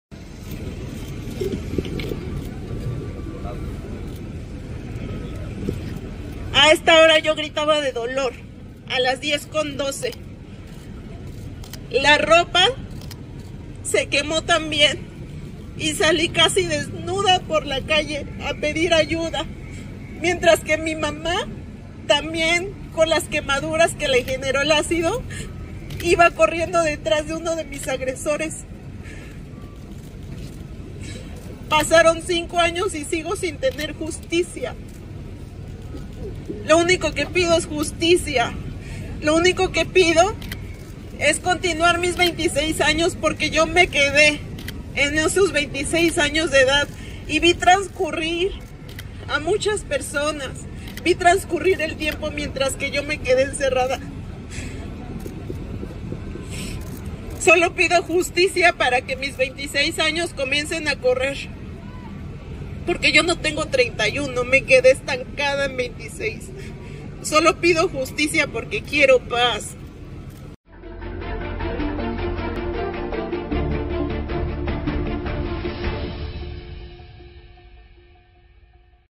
A cinco años de mi agresión no permitiré la impunidad: Elena Ríos
María Elena Ríos dijo que su familia siguen recibiendo intimidaciones del ex diputado priista, Juan Antonio Vera Carrizal. Tras cinco años del ataque con ácido que sufrió, la saxofonista exige justicia para que su caso no quede impune.